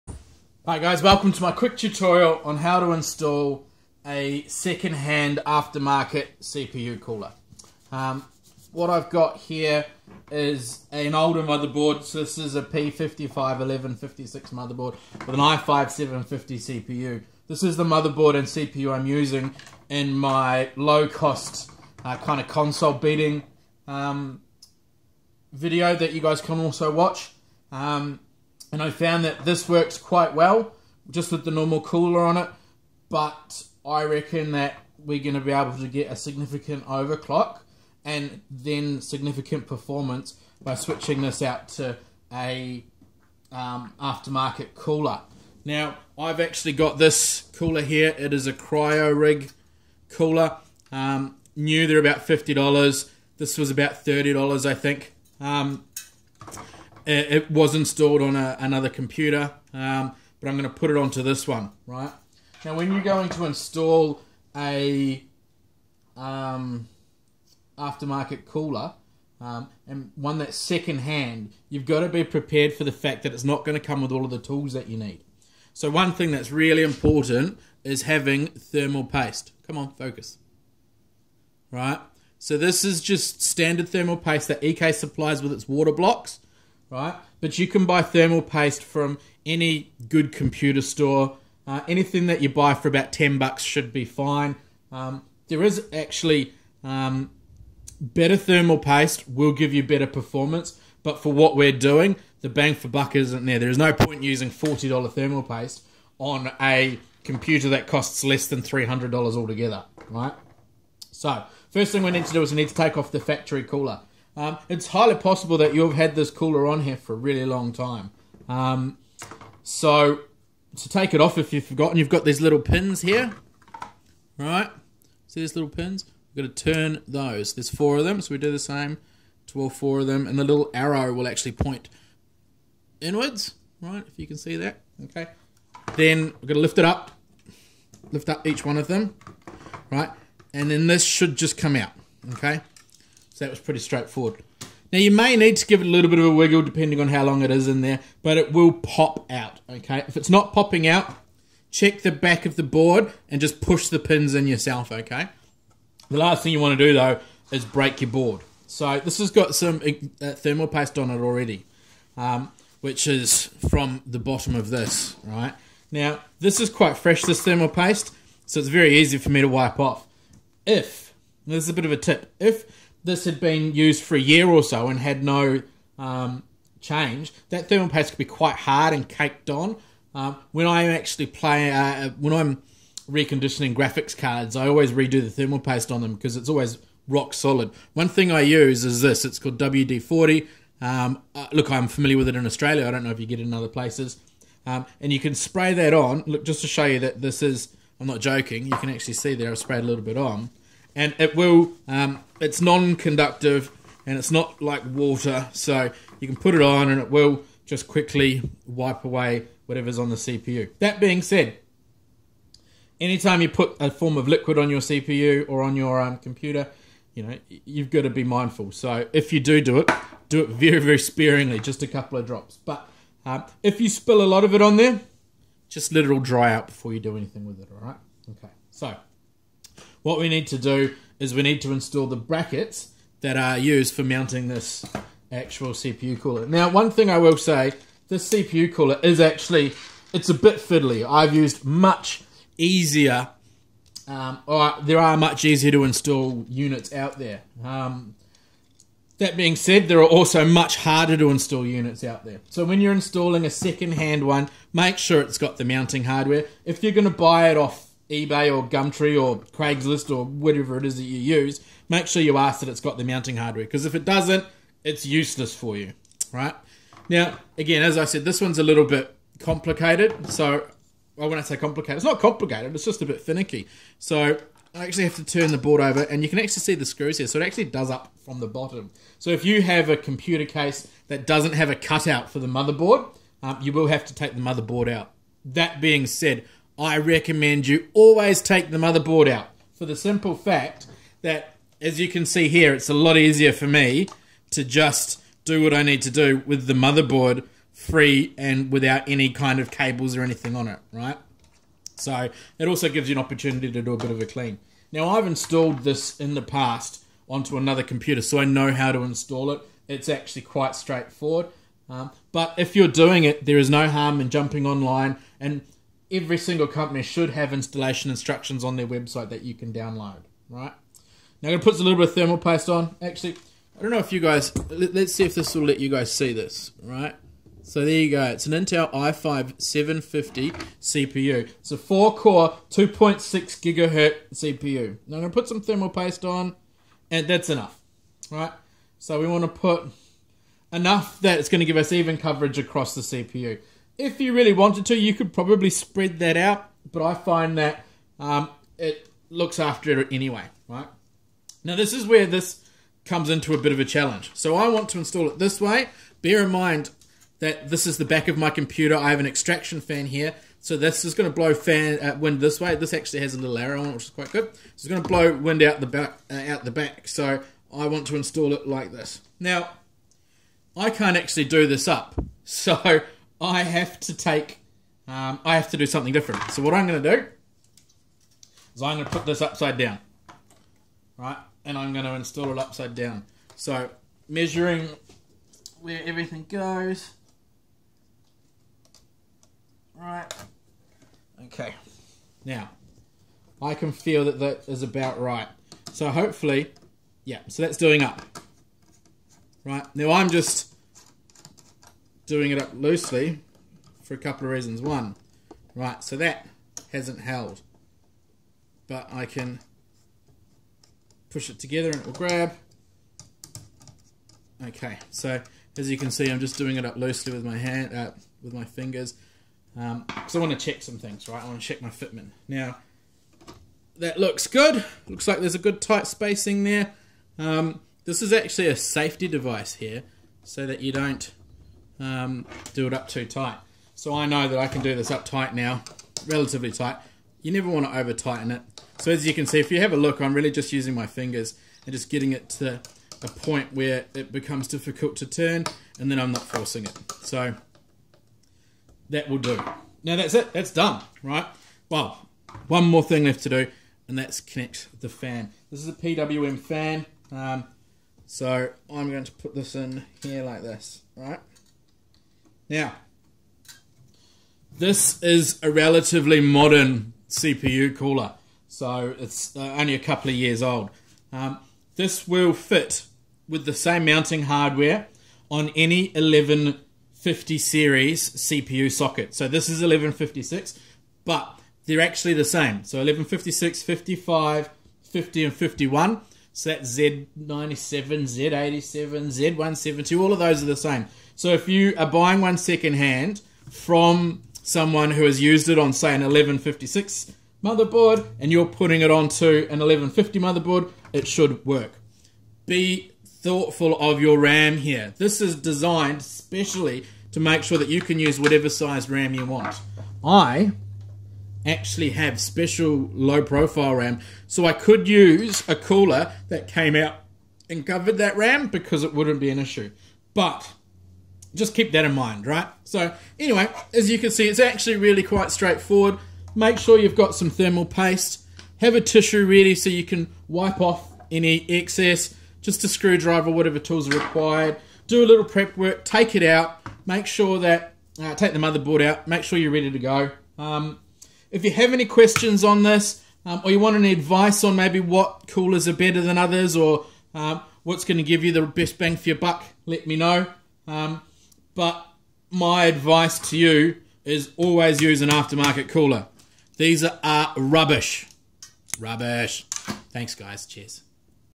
Installing a Second Hand (USED) CPU Cooler
A tutorial on what you will need, and what to look out for if you are going to install a previously used cpu cooler.